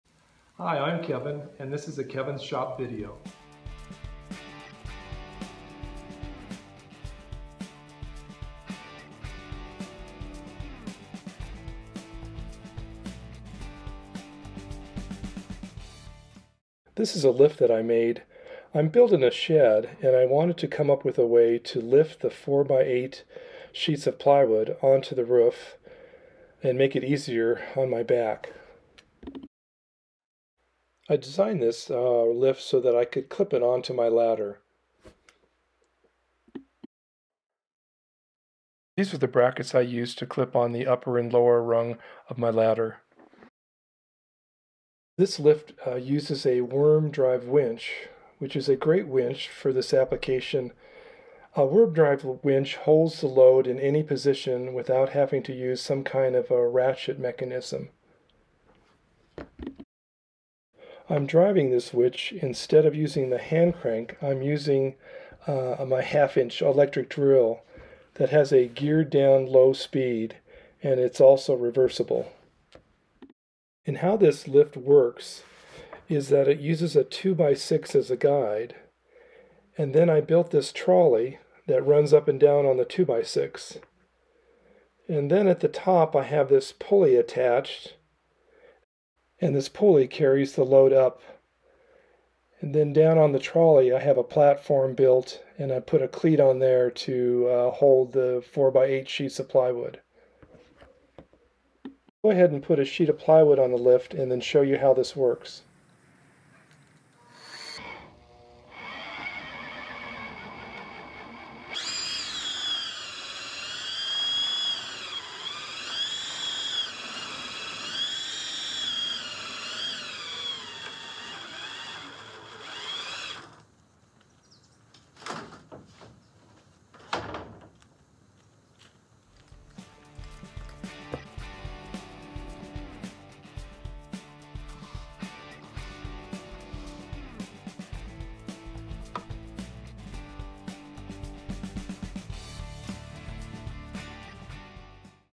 Homemade Electric Lift that attaches to my ladder!
Homemade electric lift that attaches to a ladder and easily lifts 4' x 8' sheets of plywood up to my the roof of my shed project. Uses a 1/2" electric drill to power a simple worm drive winch.